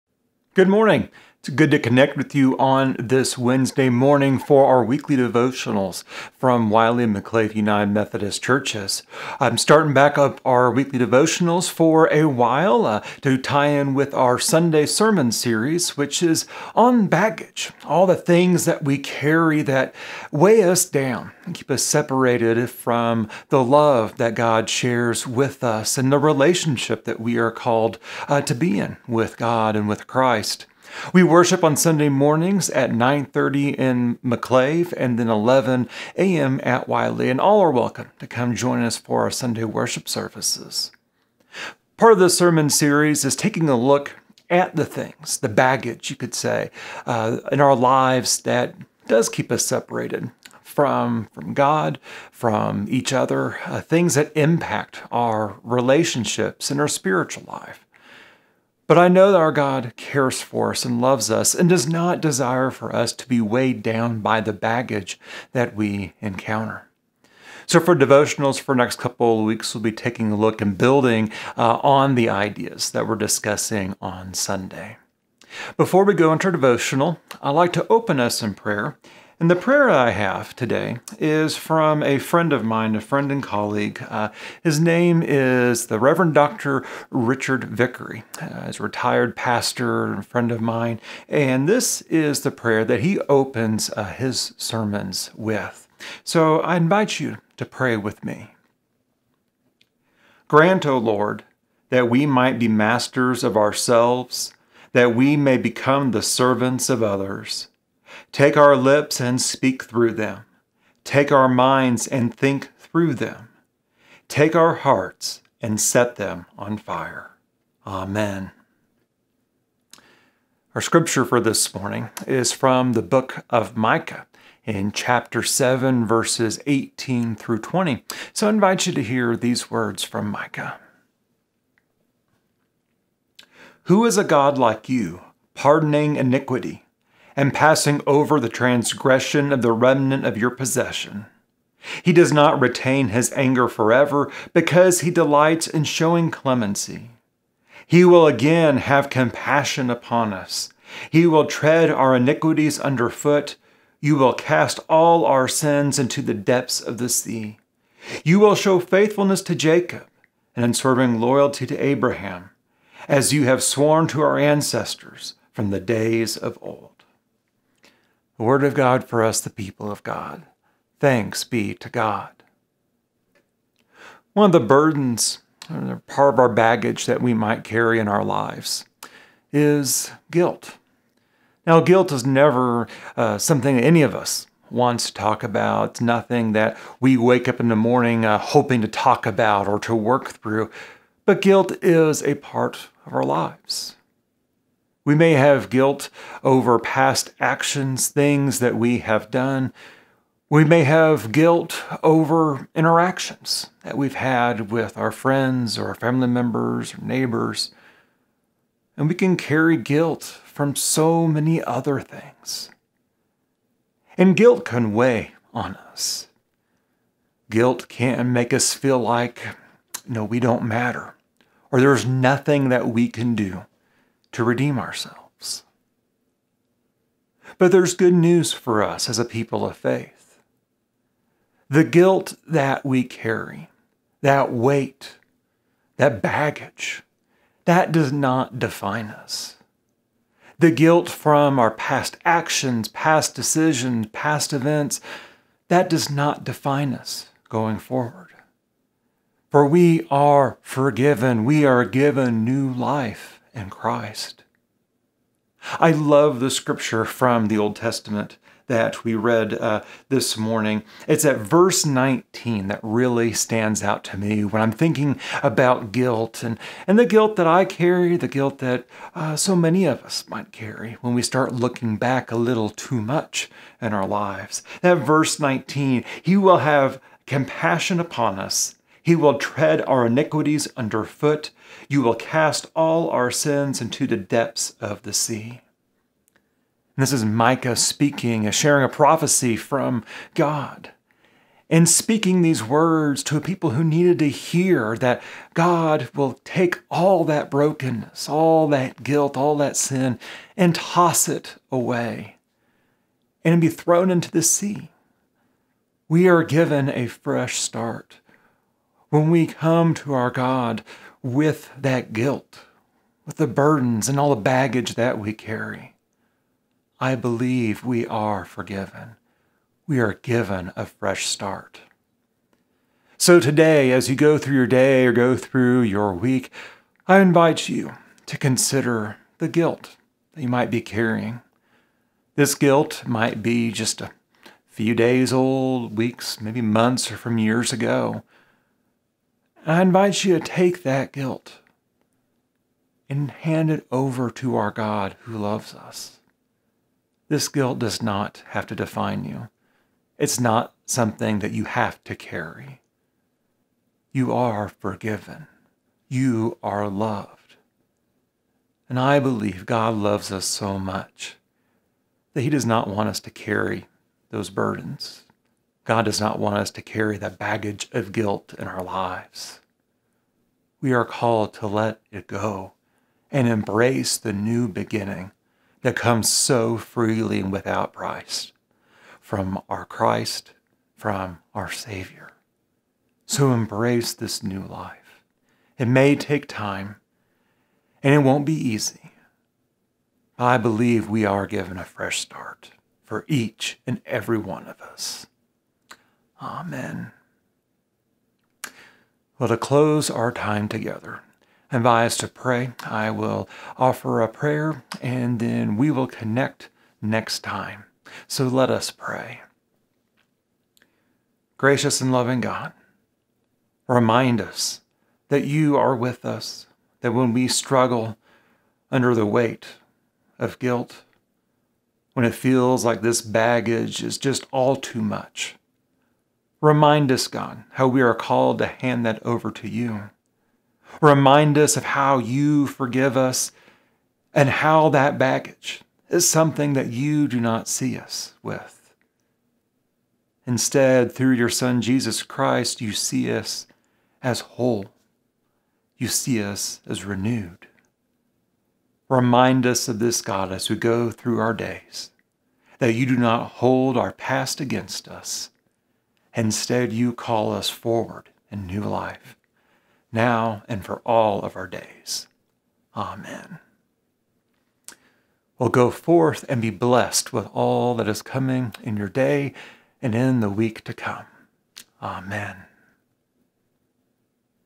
Wednesday Devotional, June 16, 2021: Guilt, Micah 7:18-20
This week's Wednesday Devotional builds on our June sermon series at McClave and Wiley United Methodist Churches. We take a look at the guilt we are carrying and how we are called not to let that define us as followers of Christ. The scripture in focus is Micah 7:18-20. Come join us for worship on Sunday at McClave United Methodist Church in McClave at 9:30am and Wiley United Methodist Church in Wiley at 11:00am.